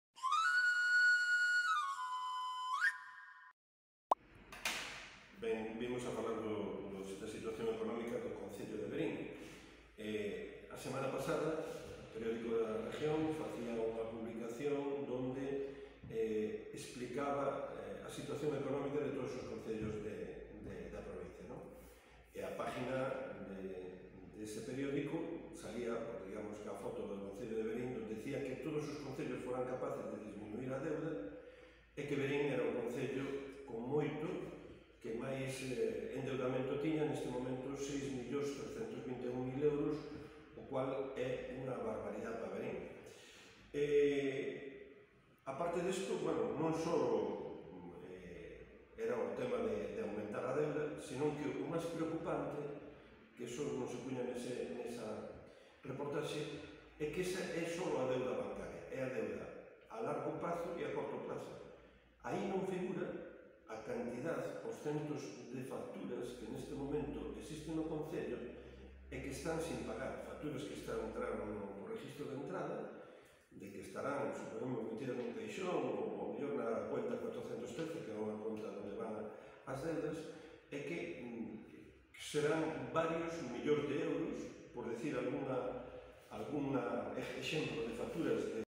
Los populares de Verín critican la situación ecónomica del Concello
El Partido Popular de Verín ha criticado en una rueda de prensa la situación económica que atraviesa el Concello de Verín, la agrupación alerta acerca del endeudamiento que sufre y critica que aun no se hayan aprobado los presupuestos de 2022. También están preocupados por la situación de Intervención del Concello.